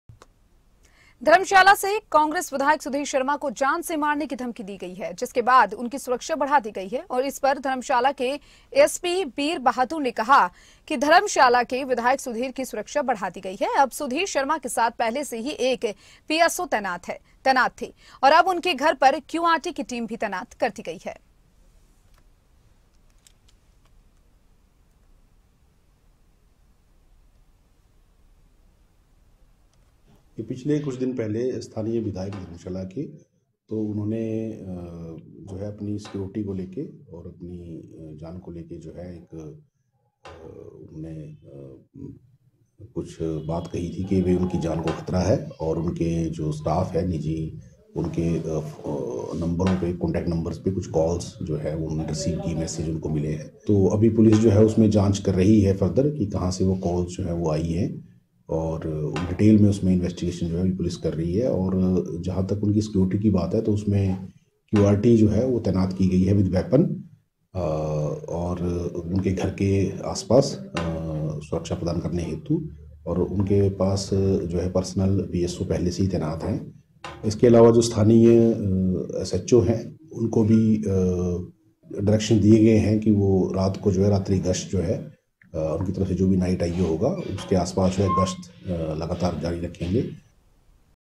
Himachal: धर्मशाला विधायक सुधीर शर्मा को मिली जान से मारने की धमकी, बढ़ाई गई सुरक्षा व्यवस्था
Himachal: धर्मशाला विधायक सुधीर शर्मा को मिली जान से मारने की धमकी
विधायक की बढ़ाई गई सुरक्षा व्यवस्था, घर पर तैनात की गई QRT